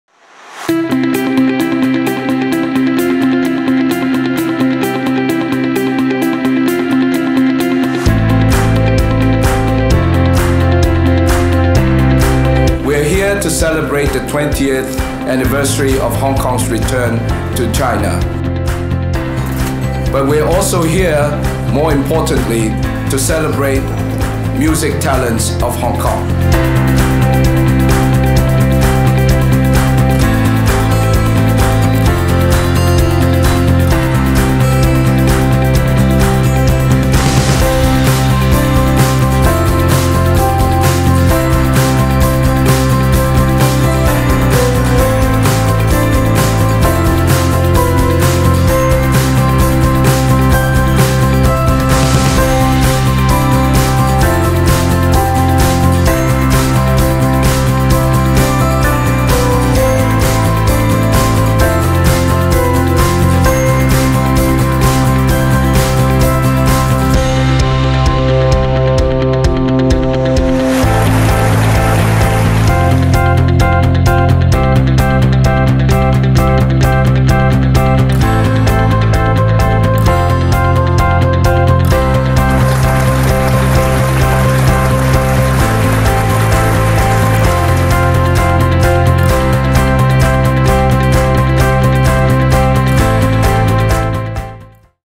Highlights of "Hong Kong Music Series" in London (Short Version)
有賴您們的支持，由香港藝術發展局主辦的「香港音樂系列」已於今年七月在倫敦順利舉行！我們衷心感謝所有曾出席和支持「香港音樂系列」五個音樂製作的您，並希望我們所挑選的製作能讓您感受到香港充滿活力和獨特的音樂文化。讓我們一同回顧一下這次音樂之旅！

With all your support, the "Hong Kong Music Series" organised by Hong Kong Arts Development Council  in London this July has ended in success! Our sincere thanks to all who had come and supported our concerts and we hope that you have enjoyed the 5 productions which were specially selected to show you the vibrant and unique musical culture of Hong Kong. Let's give this journey a quick revisit!